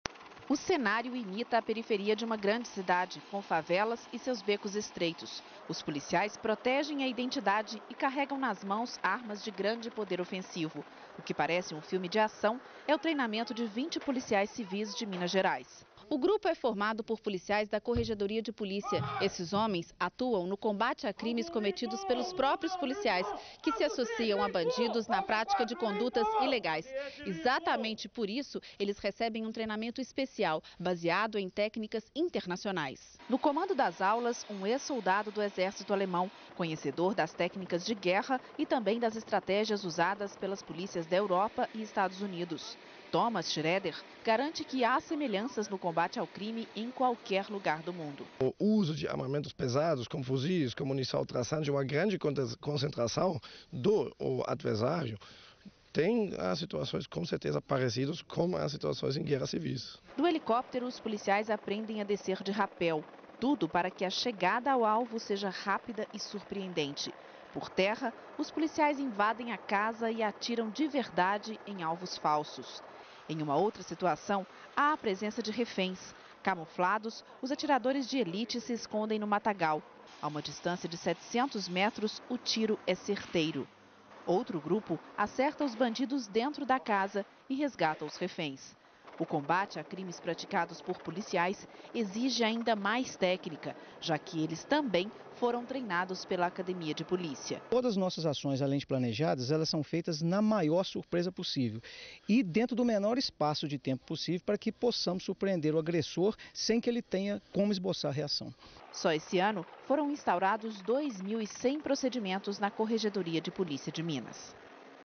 CETTAS NA REDE - TV
Report about Tactical Course at Centre for Tactical Training and Security Brazil.
Reportagem sobre treinamento Policial no CETTAS BRASIL.